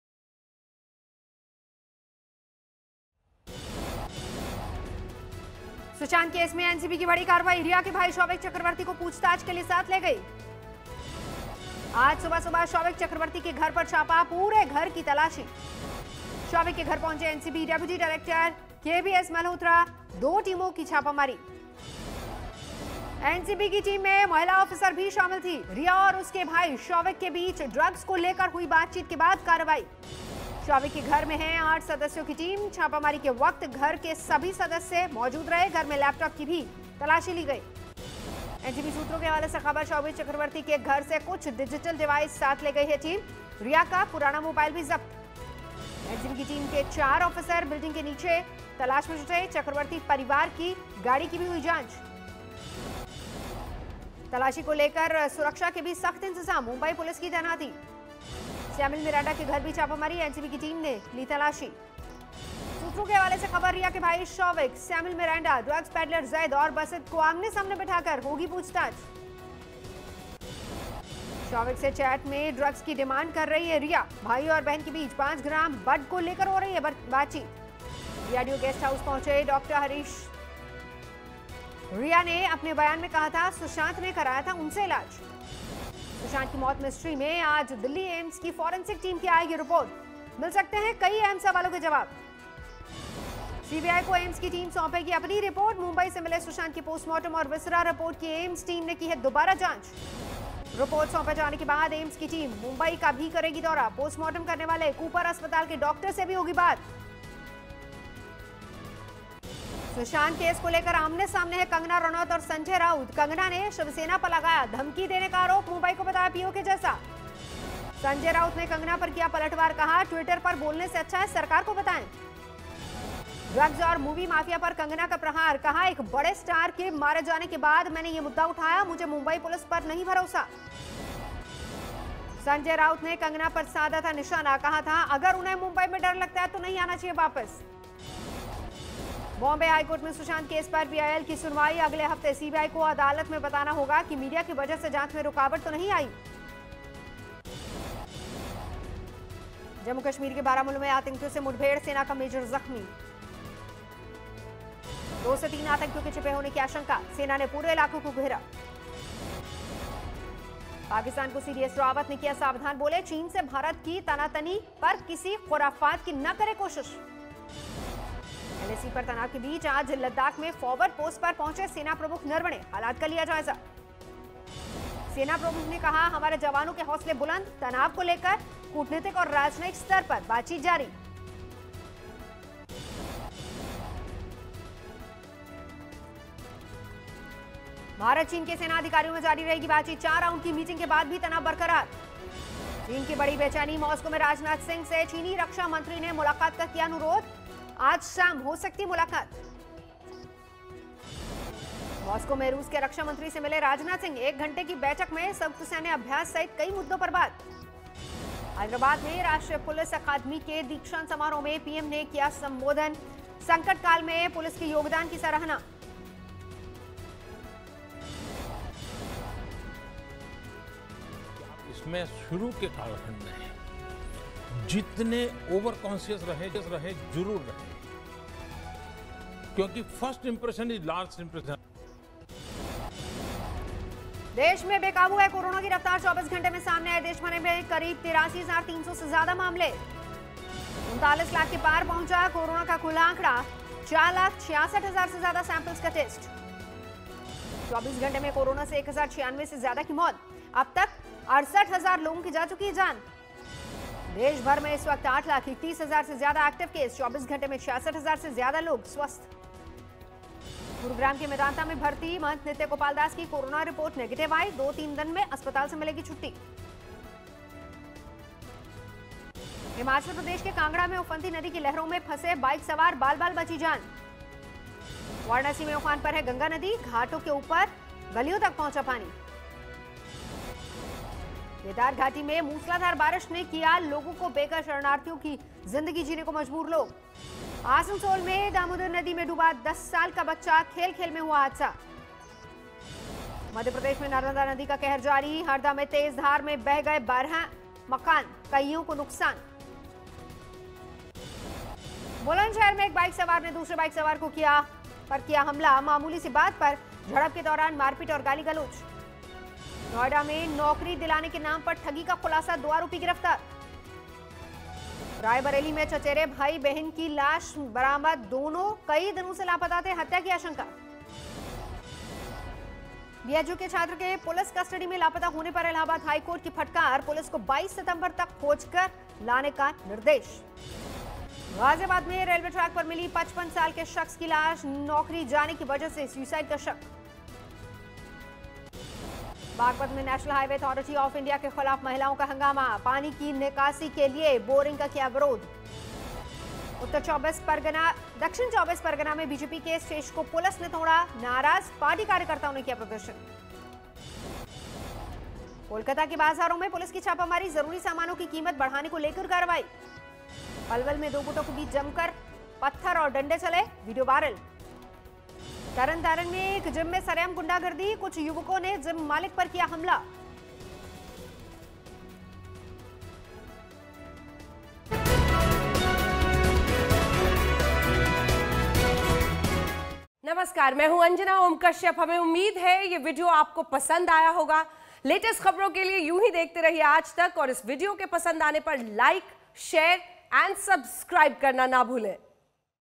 देश-दुनिया की इस वक्त की 50 बड़ी खबरें I 10 Minute 50 Khabaren I Top 50 I Sep 4, 2020
एनसीबी की टीम ने रिया के घर का सर्च ऑपरेशन खत्म किया. करीब 4 घंटे तक रिया के घर पर एनसीबी की रेड चली. एनसीबी की टीम रिया के भाई शोविक को अपने साथ पूछताछ के लिए लेकर गई है. एनसीबी की टीम रिया के भाई शोविक, सैमुअल मिरांडा, ड्रग्स पैडलर जैद और बाशित को आमने सामने बैठाकर पूछताछ करेगी. रिया के घर से फोन और बाकी इलेक्ट्रॉनिक उपकरणों को सीज किया गया है.